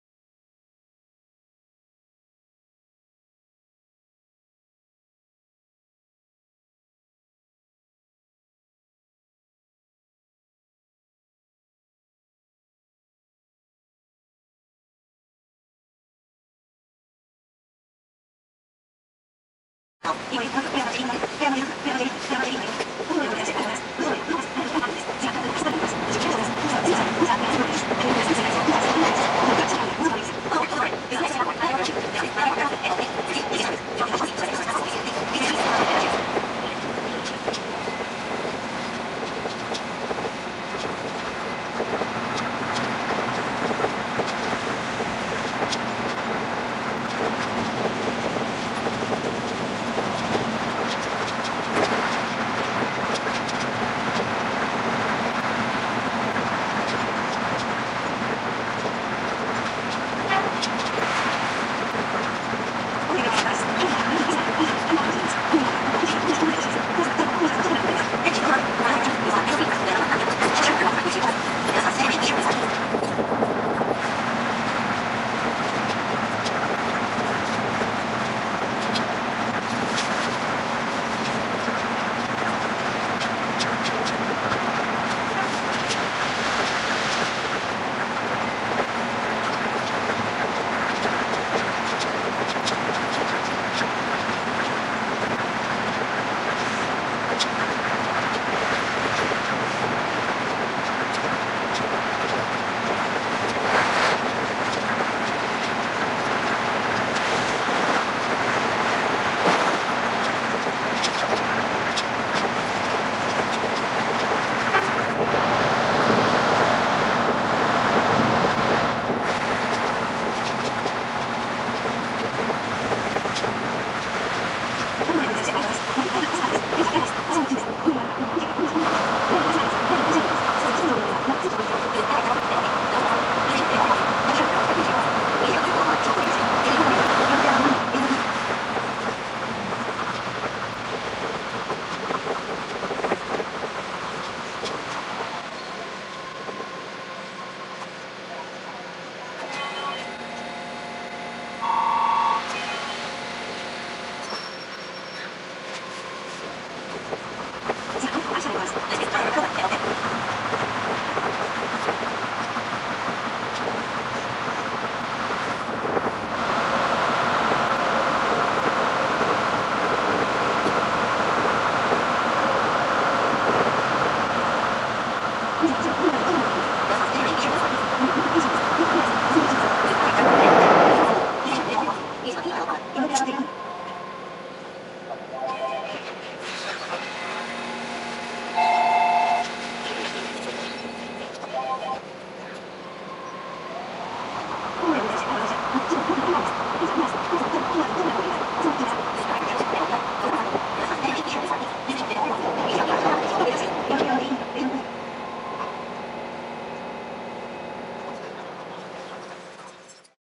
【6倍速広角車窓右側】東京モノレール空港快速浜松町〜羽田空港第2ﾀｰﾐﾅﾙ
浜松町12:30発東京モノレール空港快速羽田空港第2ﾀｰﾐﾅﾙ行きの車窓です。
乗車車両は、10000系10076です。
カメラ設置に手間取り浜松町出発後の車窓となります。